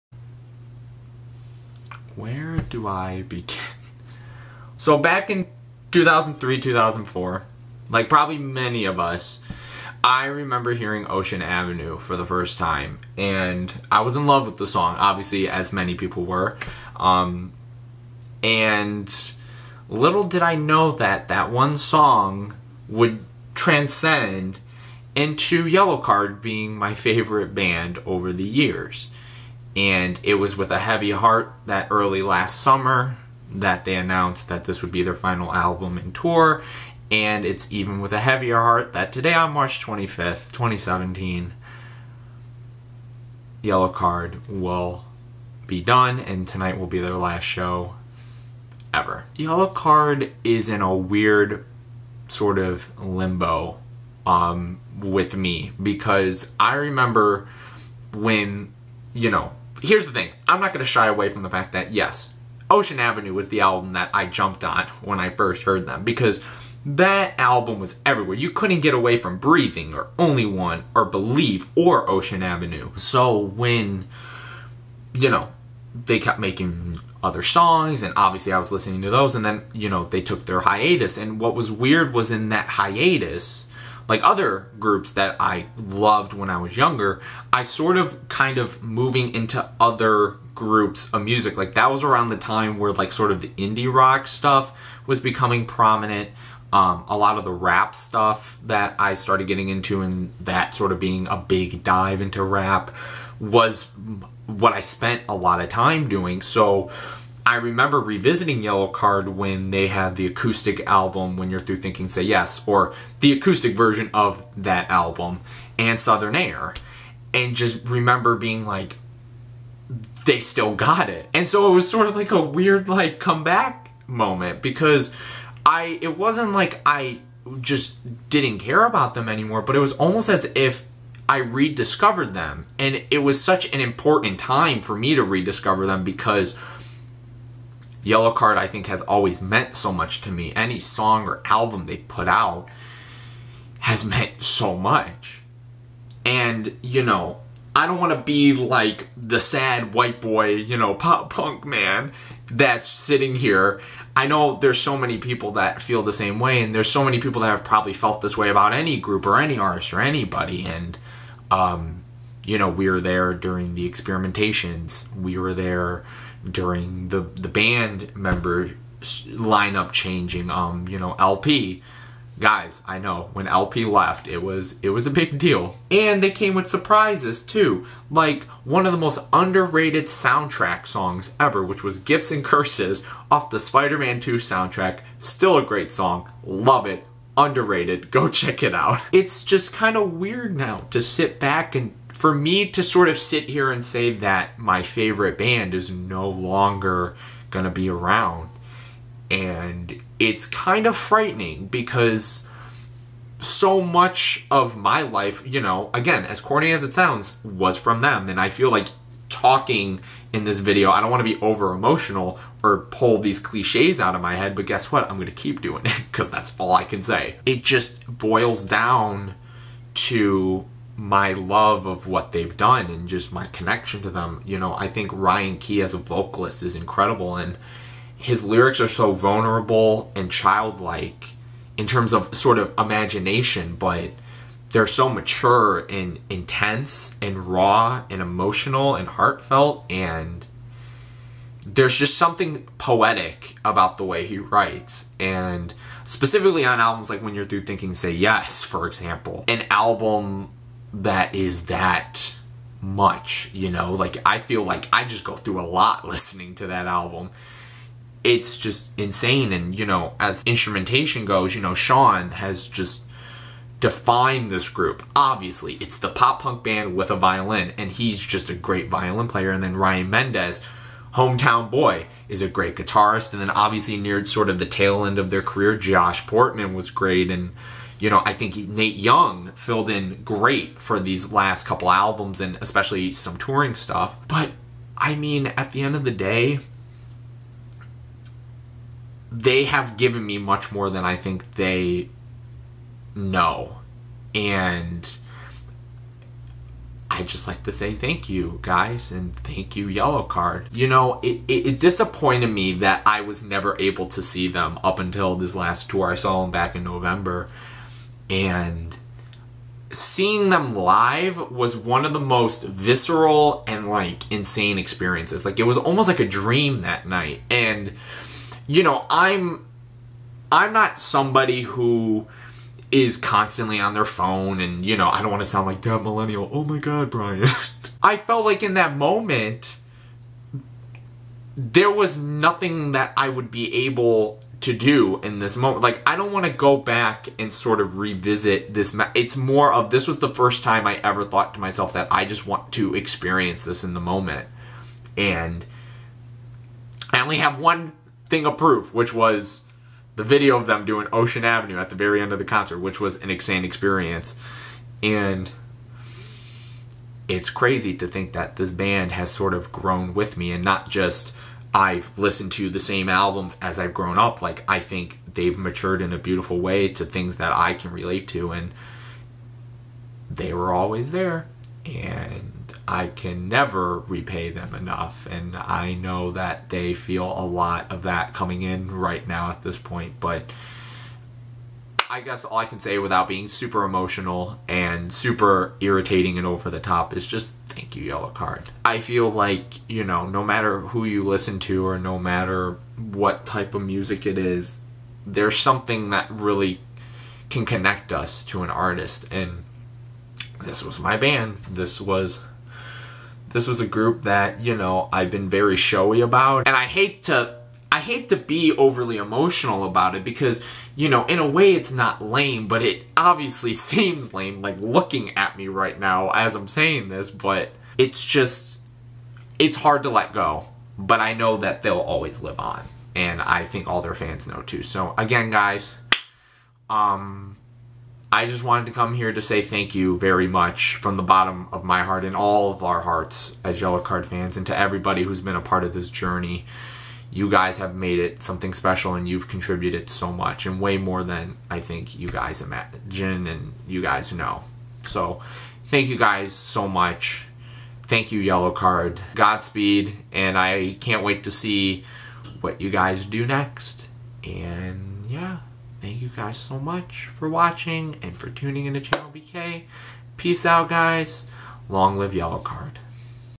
Thank You Yellowcard.
Yeah I know, I'm lame for doing a video about this but I felt like saying something okay?

Yellowcard has meant a lot to me since they commercially blew up back in the early 2000s and have stayed that way ever since. Album after album, song after song, they have influenced me in so many ways and have inspired me through all the good and bad in my life.

Since tonight on March 25, 2017 they're performing their last show, I thought that I would show how much they meant to me by showing some love with this video simply to say thank you.

To all who have been on Yellowcard's journey with me, I know it's a tough one. It's hard to sit here and have a band that meant so much to me finally take one last bow. So as Yellowcard fans, let's keep their legacy alive and share their music to more people throughout time and show them why they meant so much to us.

To all who have been involved with Yellowcard in any shape or form, thank you so very much. Ben, Todd, Warren, Ben, Pete, Alex, Sean, Nate, LP, and many more.

But most importantly thank you to Ryan, Sean, Ryan, and Josh for giving me so much to hold onto. I can't even tell you guys how much you've done for me, and you'll always continue to do so.

Thank you from the bottom of my heart Yellowcard. Thank you.

Love Now & Always,

-Brian